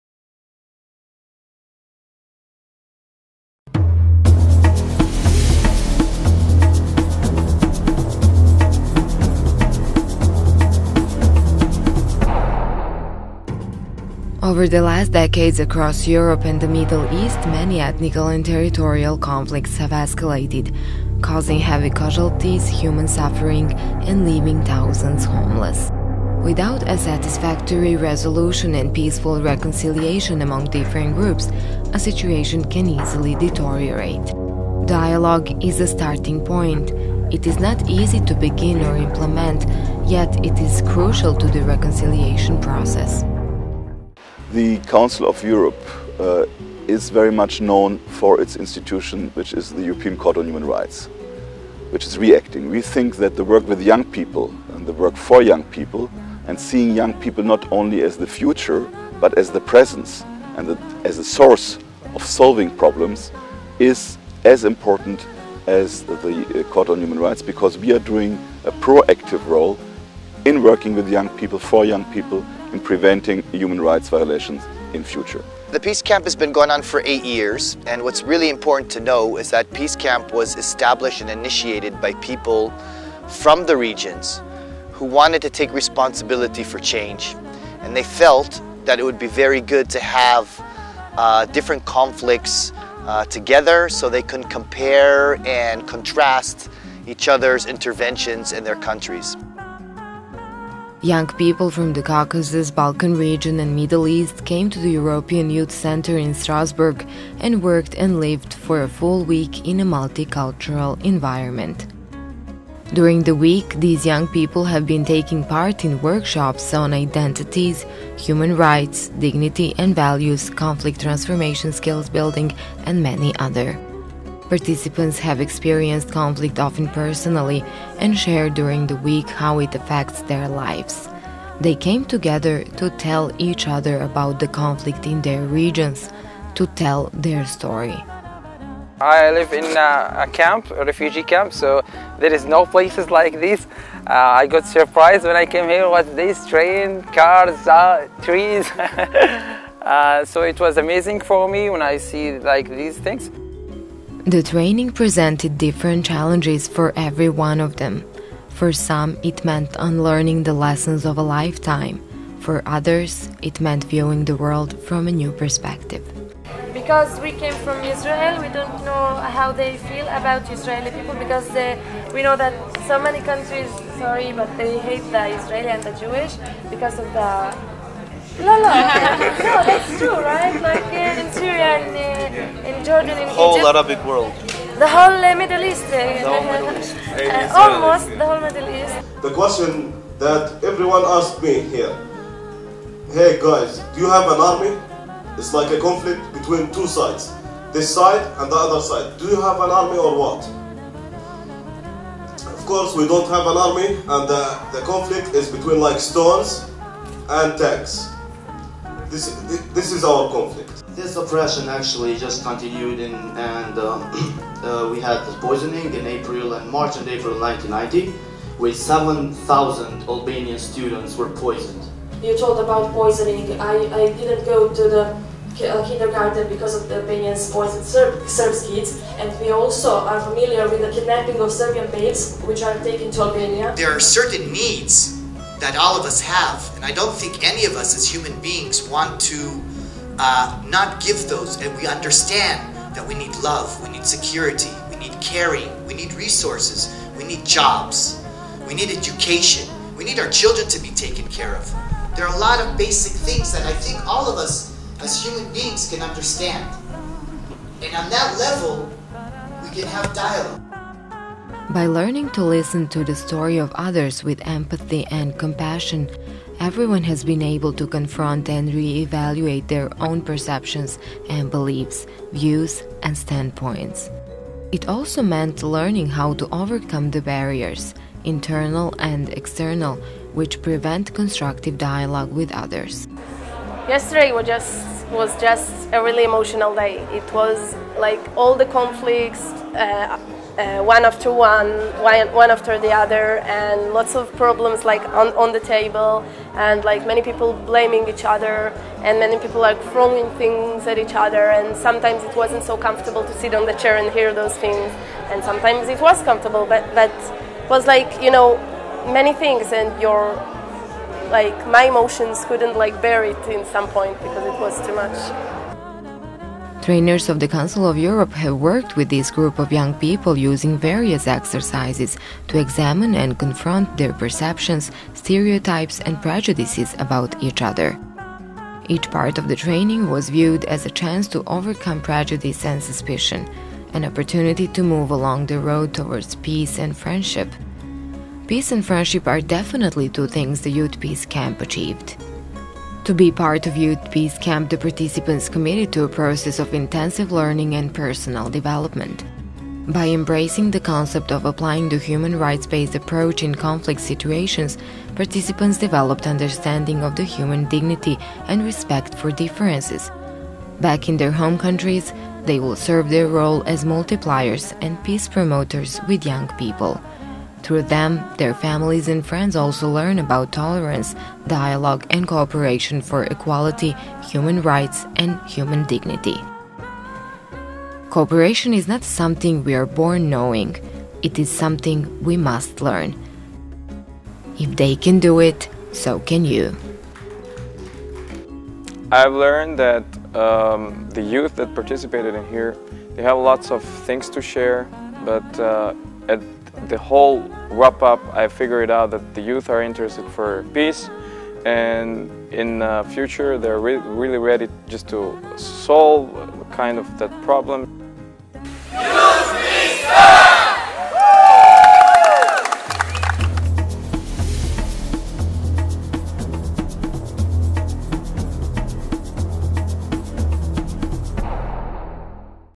Strasbourg Youth Peace Camp 2011
The 2011 edition of the Youth Peace Camp held at the European Youth Centre in Strasbourg, was a great success. The project brought together 42 young people from Armenia, Azerbaijan, Israel, Kosovo (different communities) and the Palestinian Authority allowing them to engaged in dialogue by living and working together.